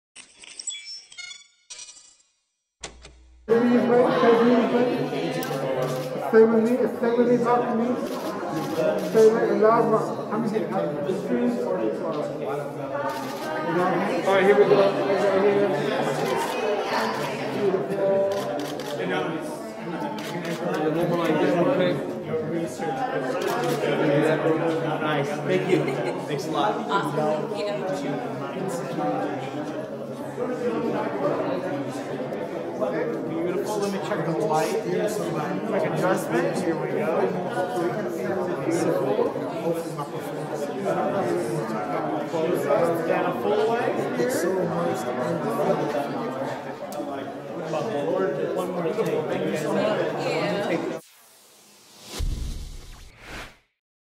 Kasia Szarek "Pleasure" Los Angeles Premiere Red Carpet
Broll footage: Actress Kasia Szarek on the red carpet at the premiere of "Pleasure" held at the Linwood Dunn Theater in Los Angeles, California USA on May 11th, 2022